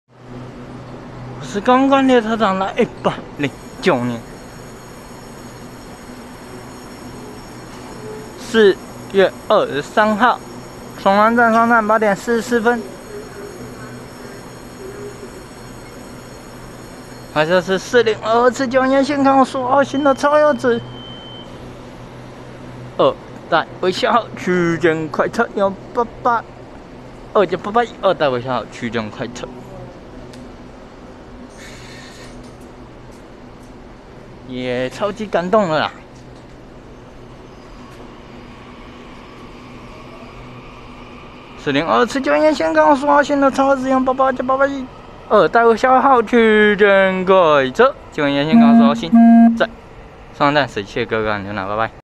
4022次經由宜蘭線開往蘇澳新的EMU882+EMU881超優質二代微笑號區間快車
109/4/23松山站8：44分拍攝4022次經由宜蘭線開往蘇澳新的EMU882+EMU881超優質二代微笑號區間快車